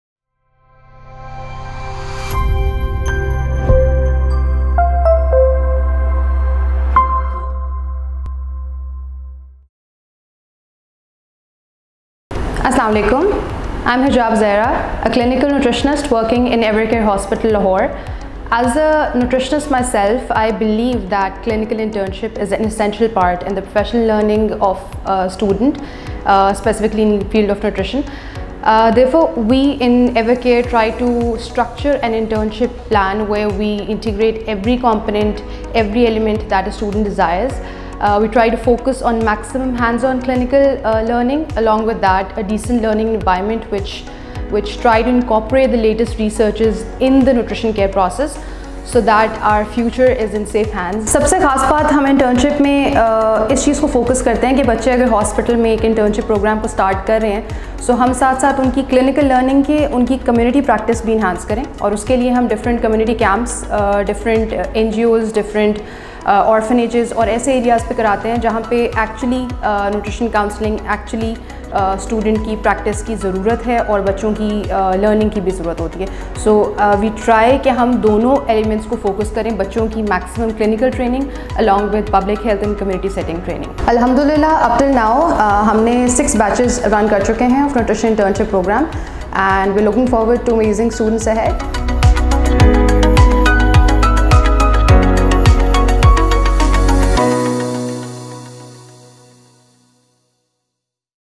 Meet Ms. Hijab Zehra Talks about experiential Nutrition Internship Program
Ms. Hijab Zehra, Clinical Nutritionist at Evercare Hospital Lahore, sheds light on the experiential Nutrition Internship Program offered at Evercare Hospital Lahore.

She talks about the importance of clinical internships for the professional learning of students, specifically in the field of nutrition.

Furthermore, she highlights the significance of a fully structured Nutrition Internship Program in which various components of the field of nutrition are integrated to provide hands-on training and clinical learning to students.